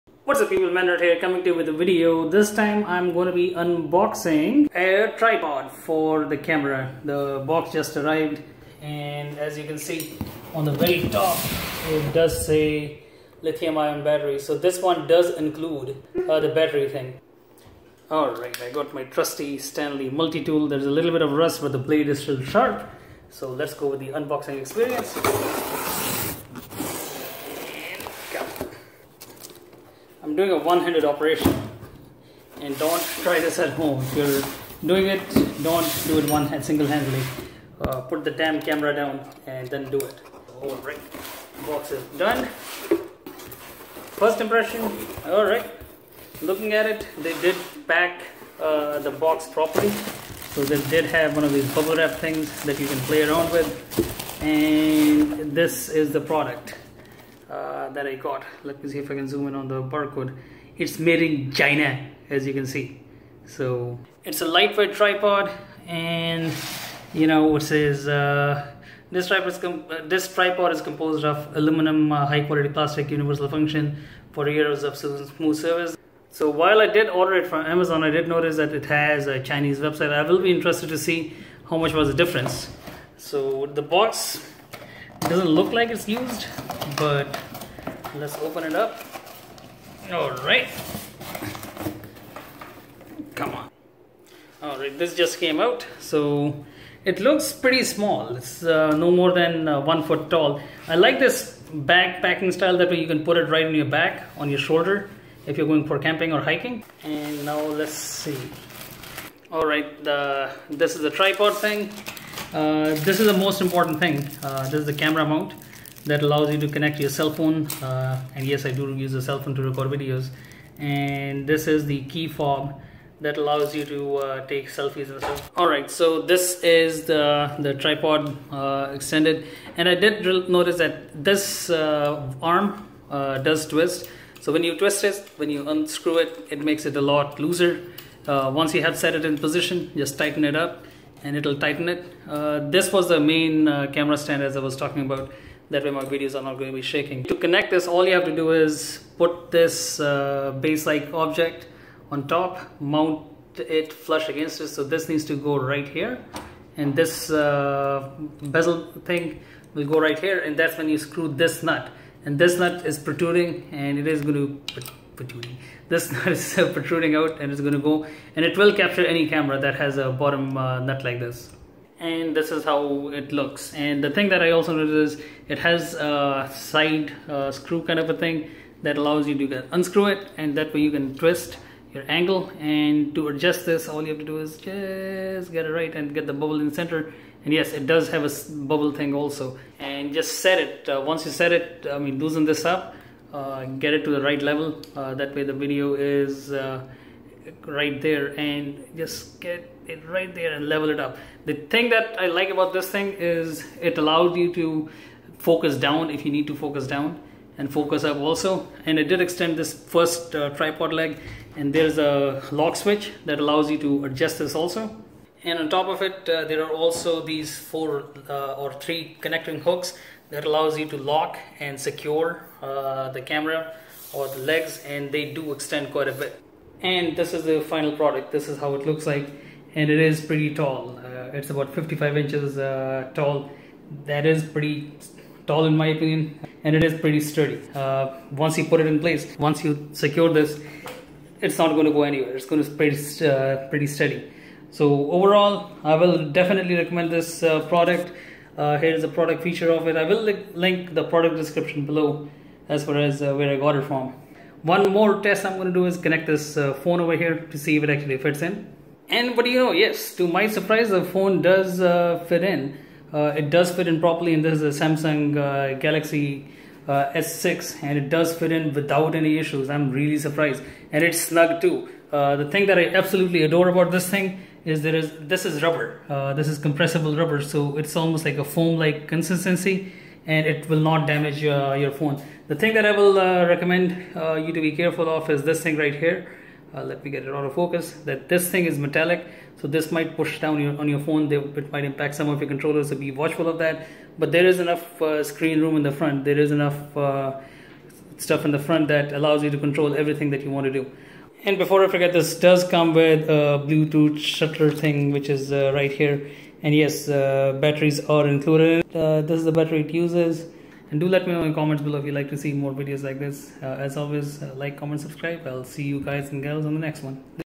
Eocean 50-Inch Tripod - Unboxing and Review
Eocean 50-Inch Tripod for Cellphone and Camera.
It does fit iPhones and Samsung device along with any camera.

Product On Amazon

Take Care